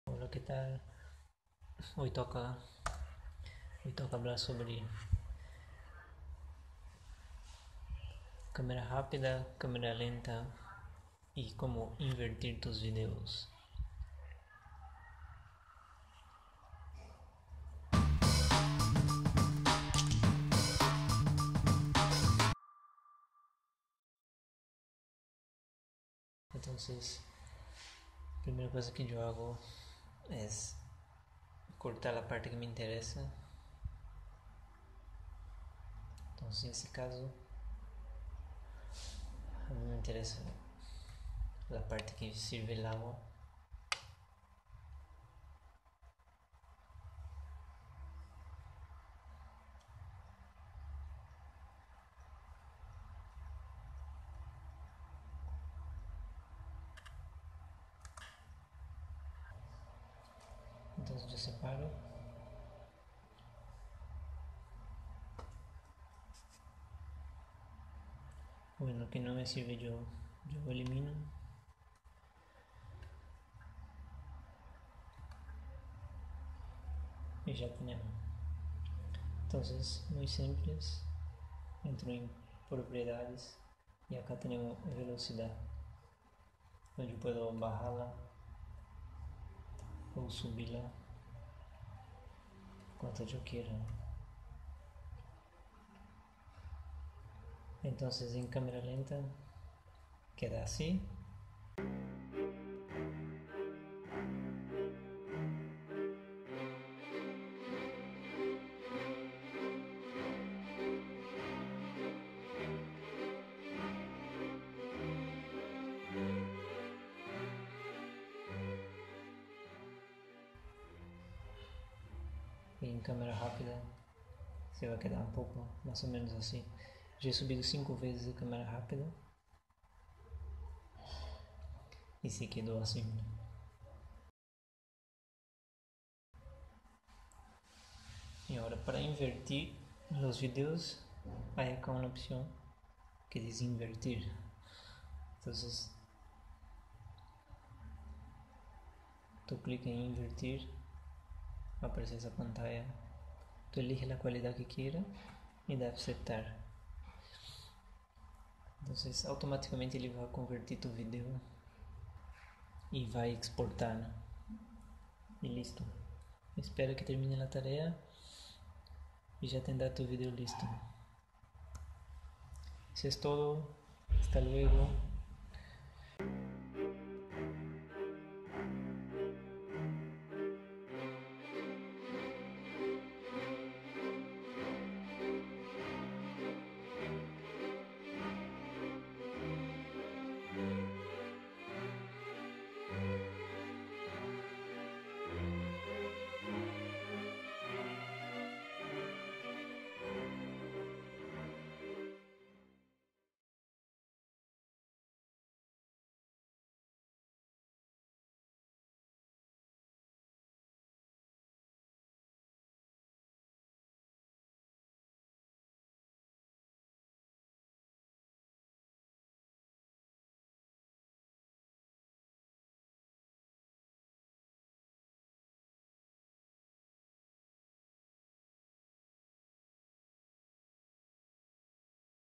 Como cambiar la velocidad y "invertir" tus vídeos en Shotcut #4
Como colocar tu vídeo en cámara rápida/lenta y modo "reverse" desde Shotcut.
Un tip es grabarlo a 60 fps para mejorar la cualidad en el momento de convertirlo.

Instagram: @vigregnol shotcut vigregnol ¿Quieres apoyarme?